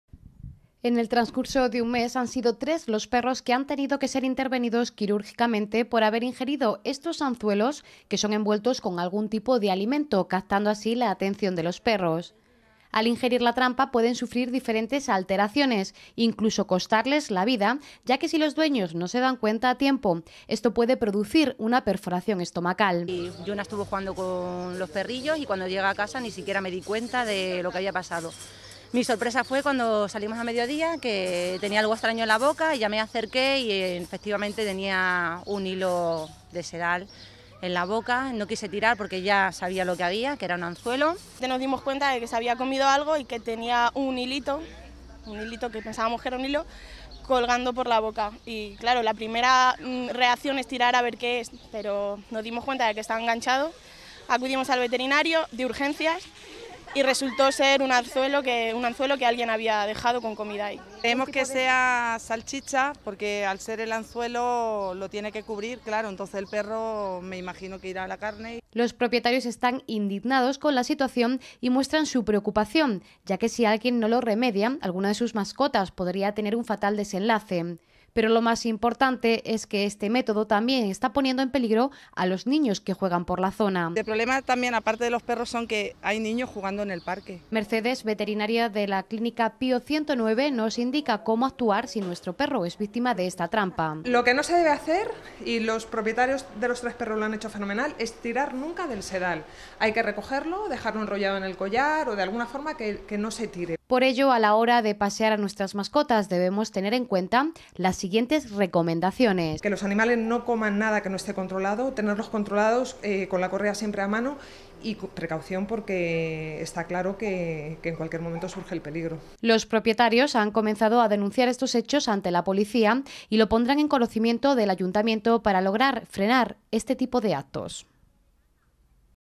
Anzuelos para perros en Talavera
Vecinos del barrio que ocupa la zona de los Santos Mártires en talavera  de la reina quieren denunciar públicamente el ataque que sus mascotas vienen sufriendo desde hace mes. Los perros son el objetivo de alguien que esconde trampas con la intención de herir a los perros poniendo en peligro sus vidas. Un anzuelo, hilo de sedal y carne son las herramientas empleadas con tal fin.